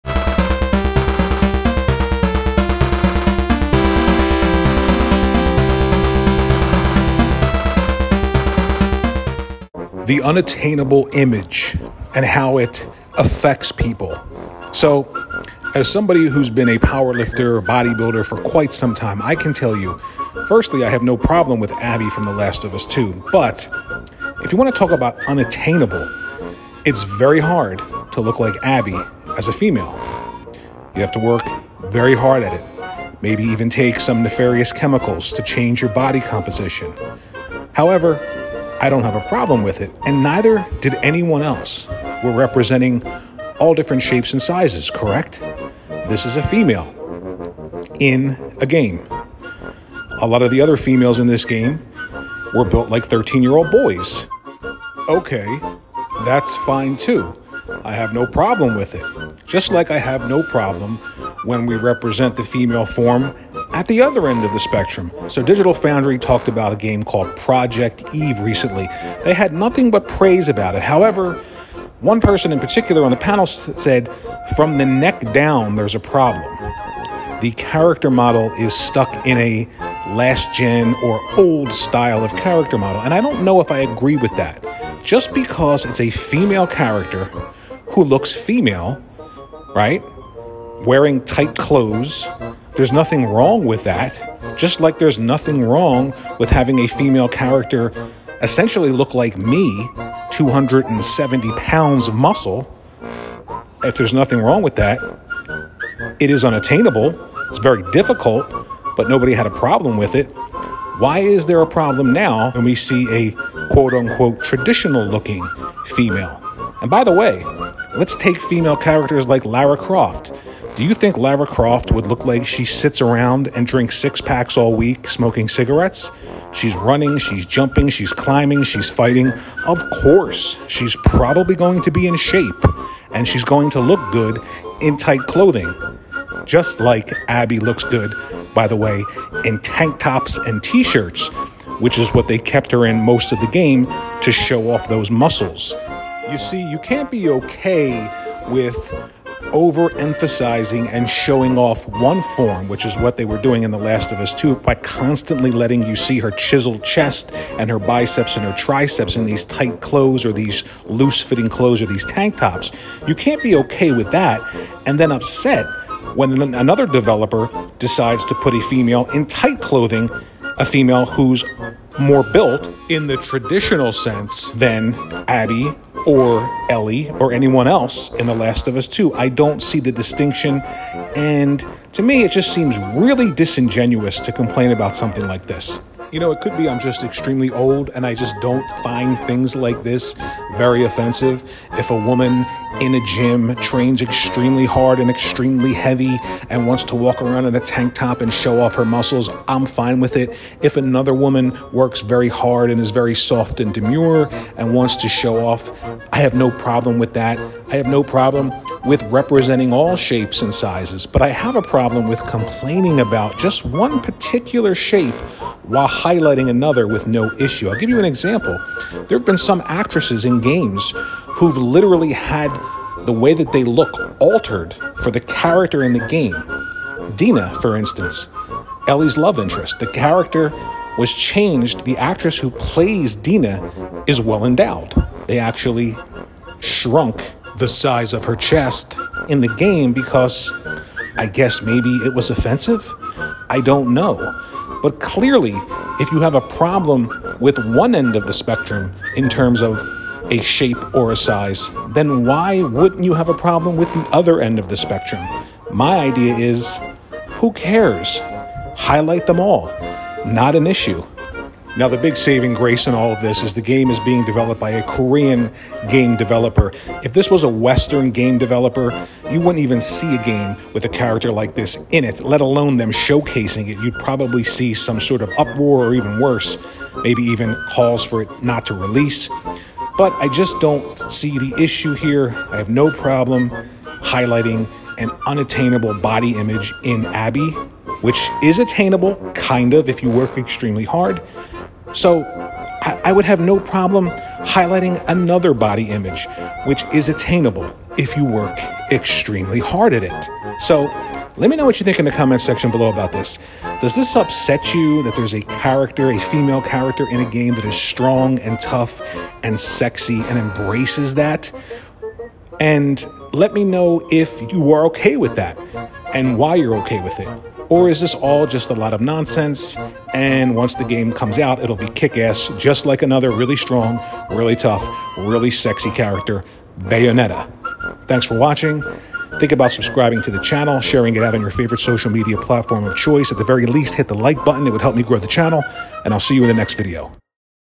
Project Eve Character Model Is An Issue? Get A Tissue
I’m all for all shapes and sizes and not for pretending one form is bad while any others are “ok”. If nobody felt upset that Abby was unattainable then why be upset that a women with a more “traditional” look is presented in a game. This is no issue at all.

XBL WorryWart73
PSN TheWorryWart73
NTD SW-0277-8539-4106